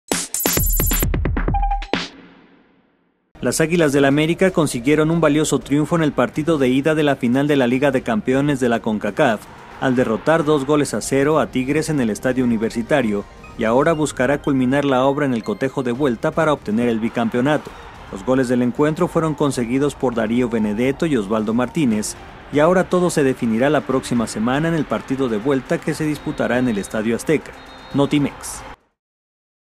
Vence América 2-0 a Tigres en ida de final de la "Concachampions"
Los goles del encuentro fueron conseguidos por Darío Benedetto y Osvalo Martínez